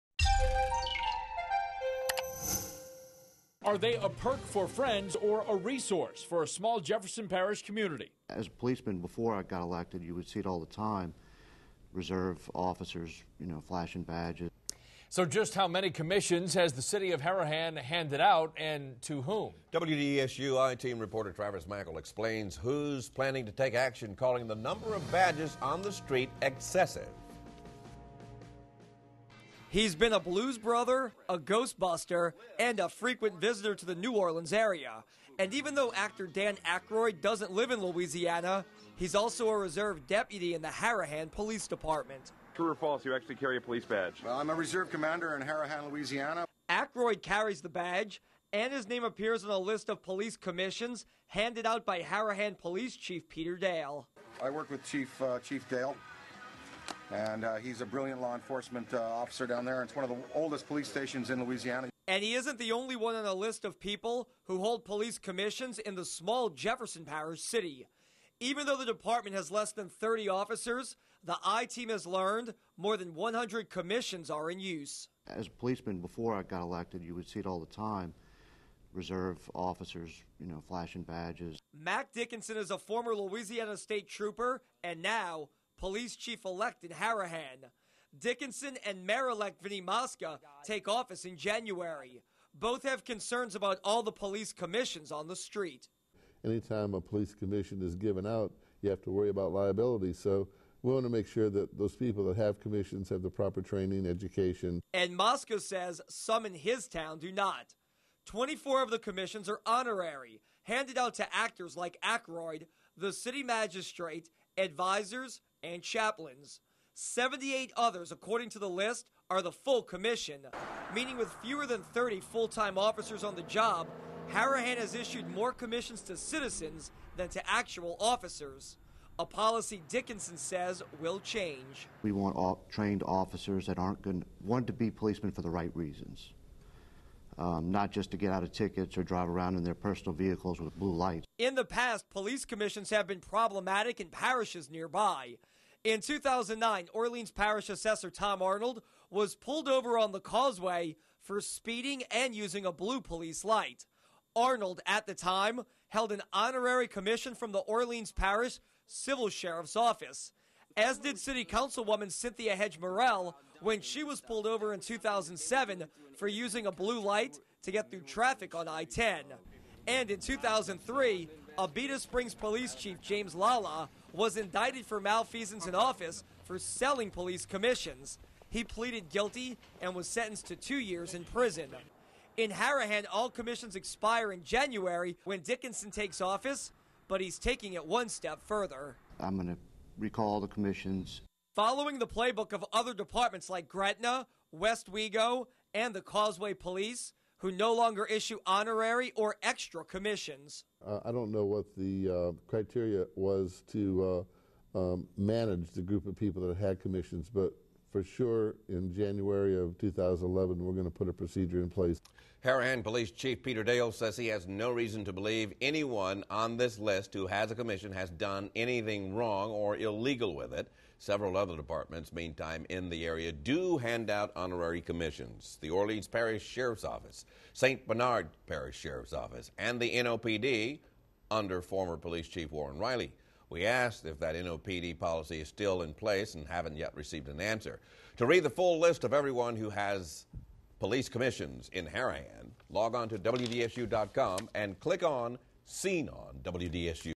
Harahan Police Commission Numbers A Concern
The number of police commission appointments in Harahan comes under scrutiny when it is learned that there are over twice as many commission appointments as there are actual working officers.